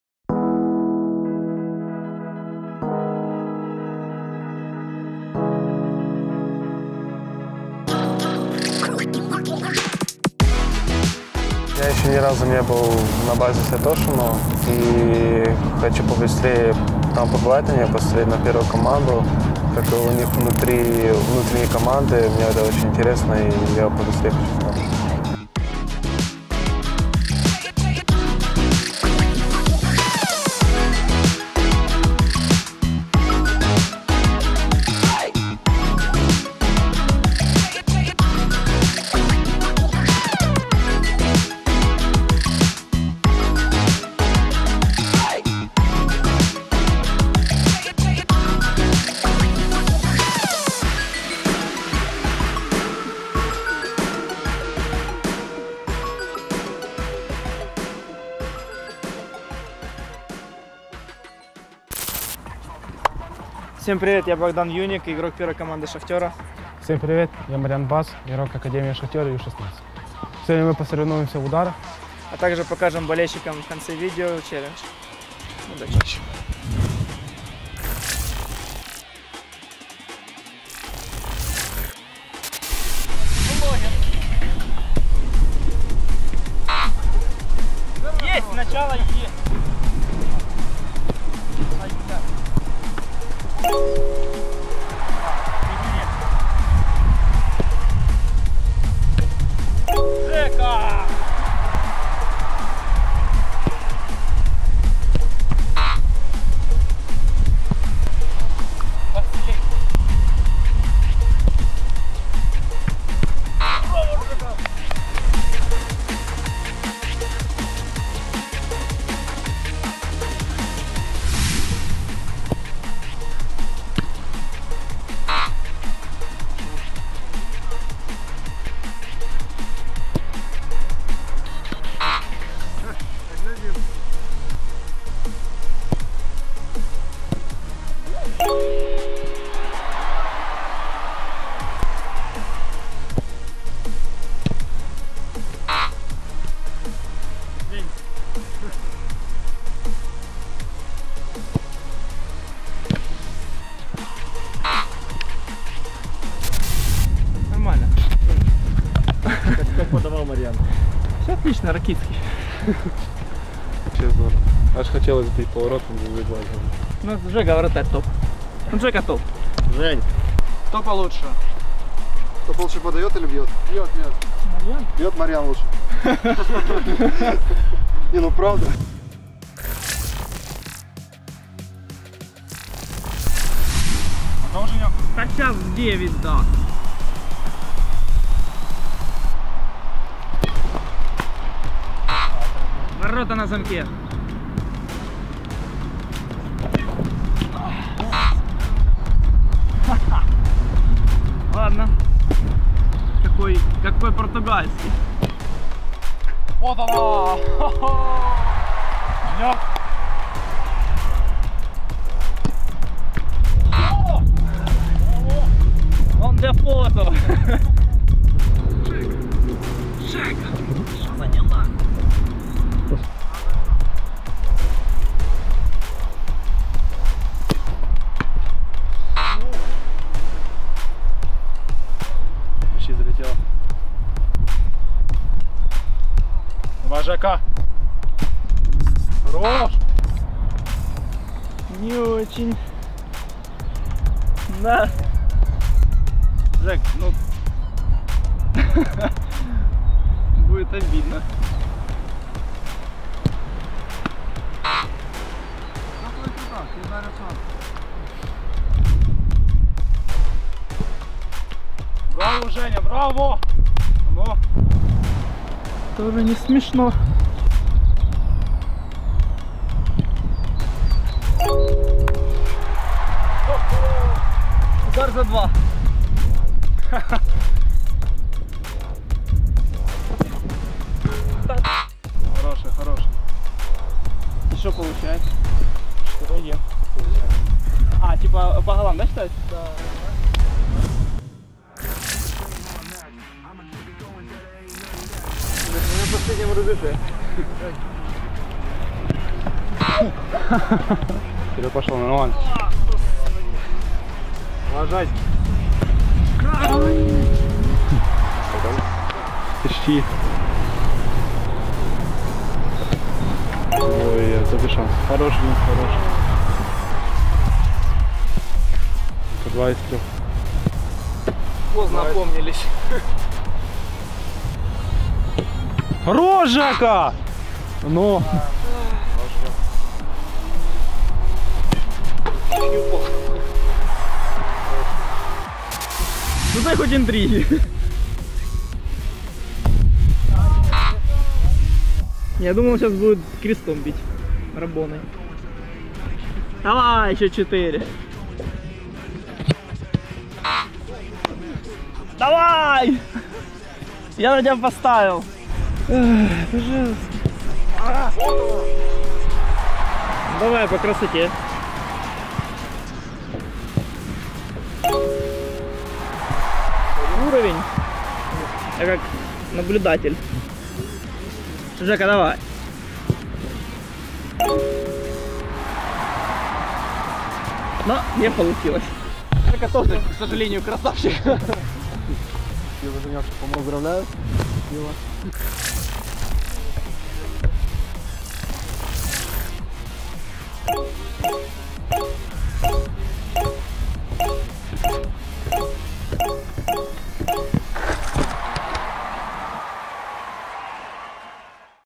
Футбольный челлендж игроков Шахтера: удары с разных позиций
Нападающий «оранжево-черных» Богдан Вьюнник и защитник «Шахтера» U16 Марьян Бац сошлись лицом к лицу в челлендже ударов. Смотрите, что из этого получилось.